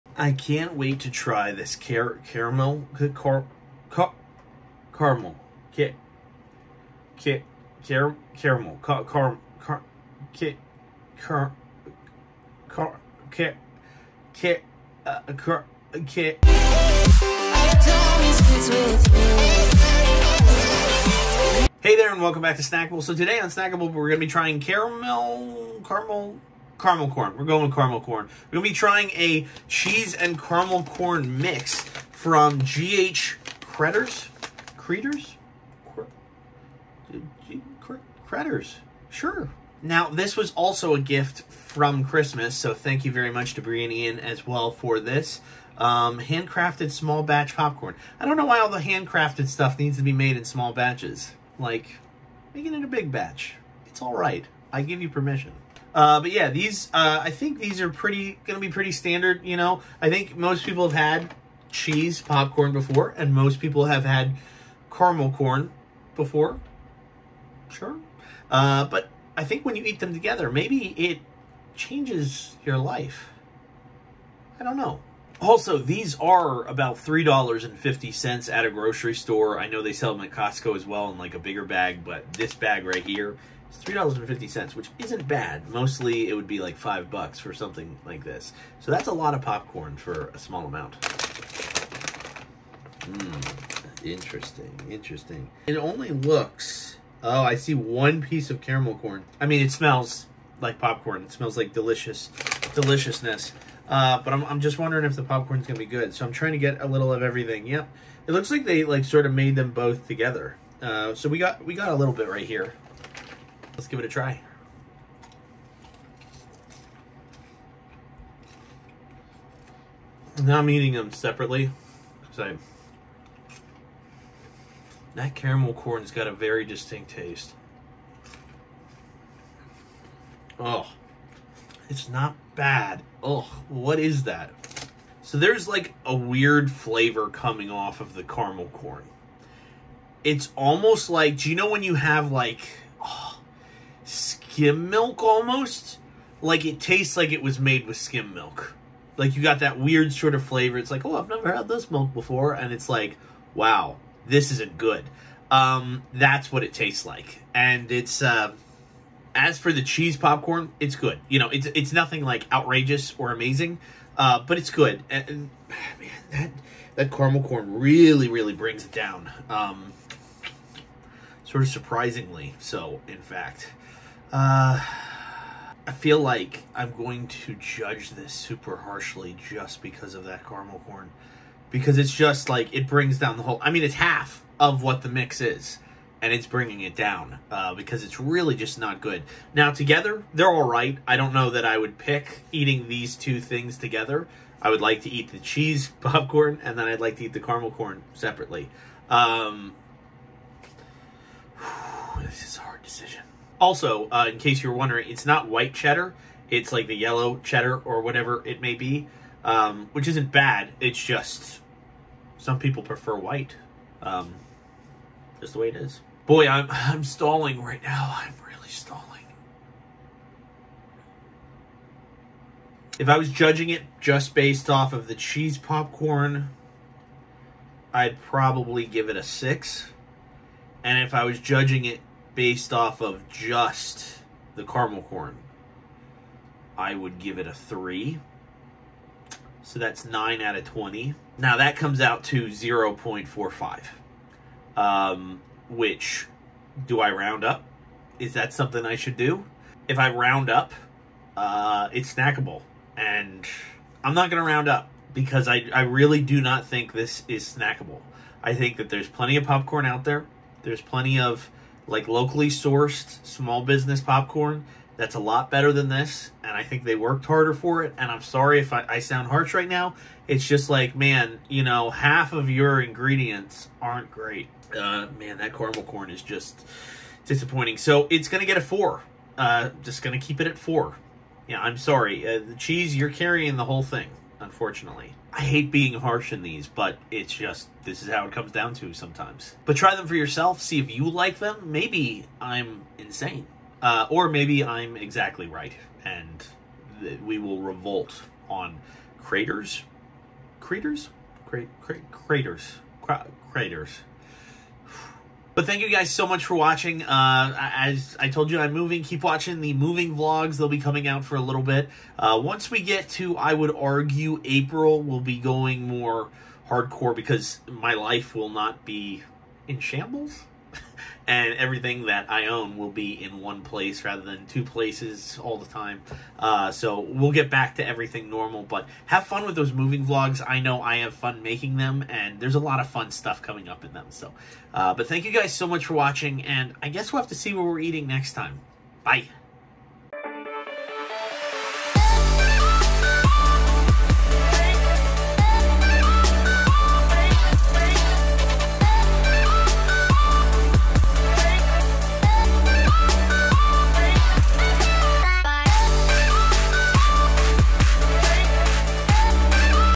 Cretors Popcorn Mix! Snackable Ep. 53
Today on Snackable we have a popcorn mix from Cretors with both Caramel and Cheese Popcorn. It's made in small batches, but is it Snackable?

Wanna keep up to date on what we are doing?